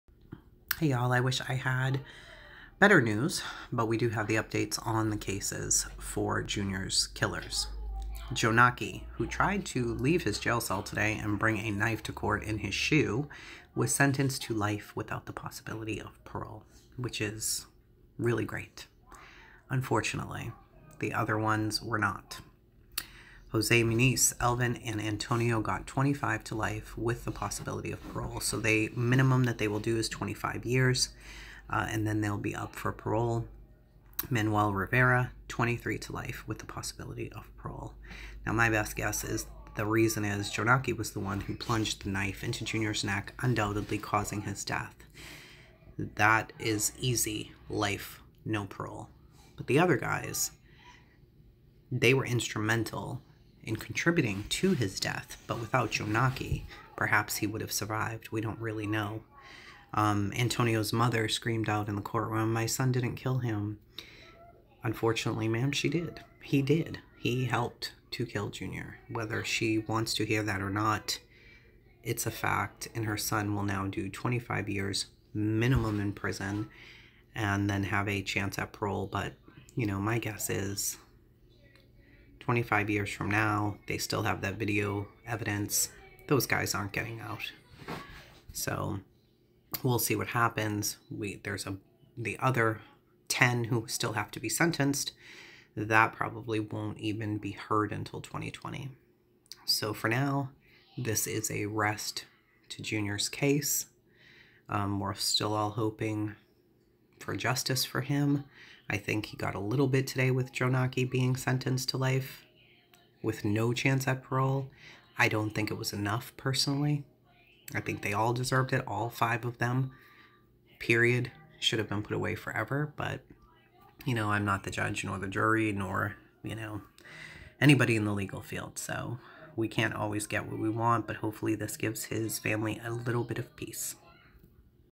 Junior Guzman Feliz case update
Jonaki Martinez was sentenced to life with no parole.  Jose Muniz, Elvin Garcia and Antonio Rodriguez were sentenced to 25 to life and finally Manuel Rivera got 23 to life.